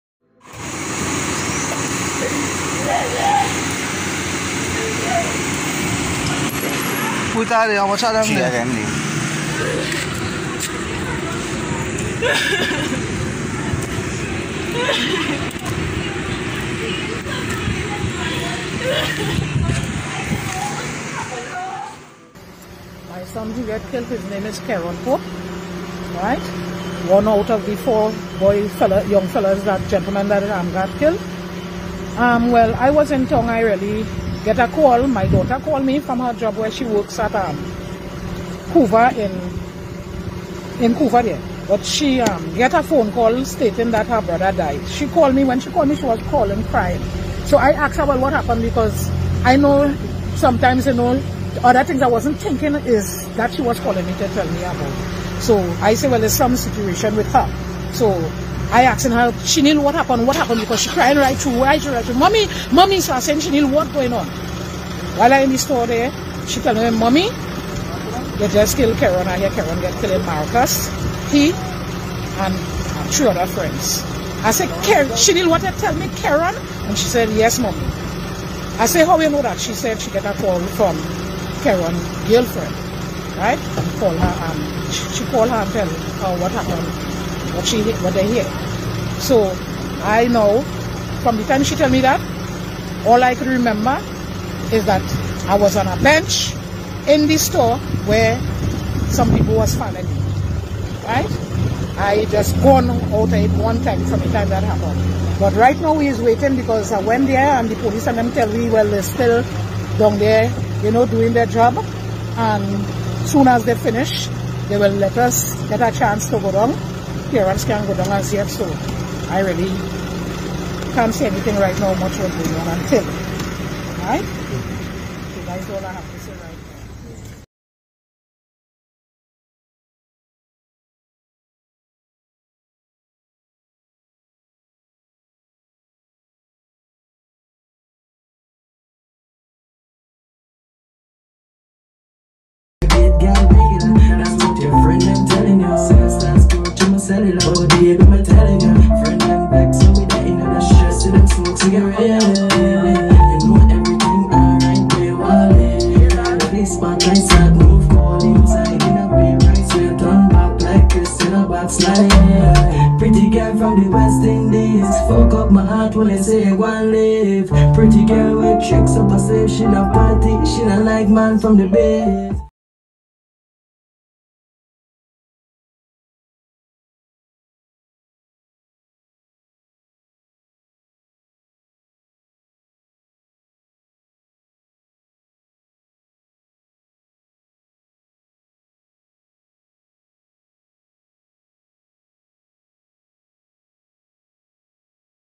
Trinibad artist Cj Dan Was Shot Dead In Maracas
Four men shot dead in Maracas

Police are on the scene of a quadruple murder at Damian Bay, Maracas.
The victims have been identified as Keron Pope, Elijah Farrel, CJ Dan and "Mincy Boy".
They all lived at Santa Cruz.

According to preliminary reports, around 12:15pm on Saturday the four men were in a car with two women exiting Damian Bay, Maracas, when a vehicle pulled up behind them.

One man exited the vehicle and fired several shots.
All four men were hit.
The women were unharmed.
This a developing story and will be updated

SOCIAL

📷 tnanp868  Add For Graphic Content